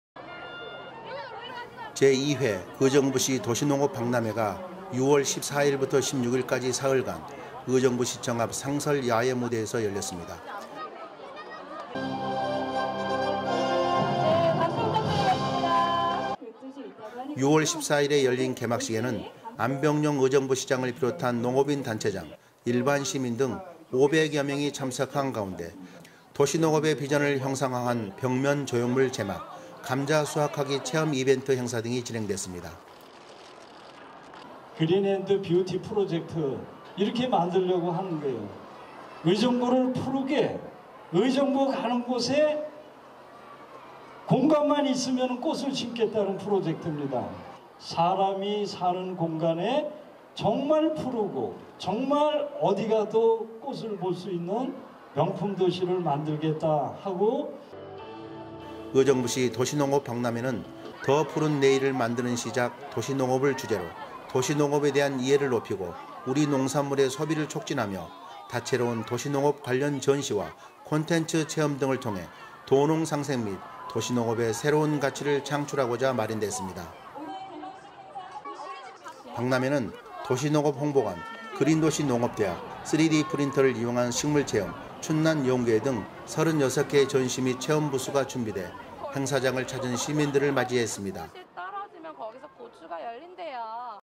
[JBC중앙보훈방송] 경기도 의정부시, ‘도시농업박람회’ 개최···‘더 푸른 내일을 만드는 시작! 도시농업’을 주제로
【중앙보훈방송=오재욱 기자】 ‘제2회 의정부시 도시농업박람회’가 6월 14일부터 16일까지 사흘간 의정부시청 앞 상설야외무대에서 열렸습니다.

6월 14일에 열린 개막식에는 안병용 의정부시장을 비롯한 농업인 단체장, 일반 시민 등 500여 명이 참석한 가운데 도시농업의 비전을 형상화한 벽면 조형물 제막, 감자 수확하기 체험 이벤트행사 등이 진행됐습니다.

[안병용 / 의정부시장]
“Green and Beauty Project 이렇게 만들려고 하는 거예요. 의정부를 푸르게, 의정부 가는 곳에 공간만 있으면 꽃을 심겠다는 프로젝트입니다. 사람이 사는 공간에 정말 푸르고 정말 어디가도 꽃을 볼 수 있는 명품도시를 만들겠다하고 .”

의정부시 도시농업박람회는 ‘더 푸른 내일을 만드는 시작! 도시농업’을 주제로 도시농업에 대한 이해를 높이고 우리 농산물의 소비를 촉진하며, 다채로운 도시농업 관련 전시와 콘텐츠 체험 등을 통해 도농상생 및 도시농업의 새로운 가치를 창출하고자 마련됐습니다.

박람회는 도시농업홍보관, 그린도시농업대학, 3D프린터를 이용한 식물체험, 춘란연구회 등 36개 전시 및 체험부스가 준비돼 행사장을 찾은 시민들을 맞이했습니다.

(영상취재 : 오재욱, 박정미 기자 / 편집 : 오재욱 기자)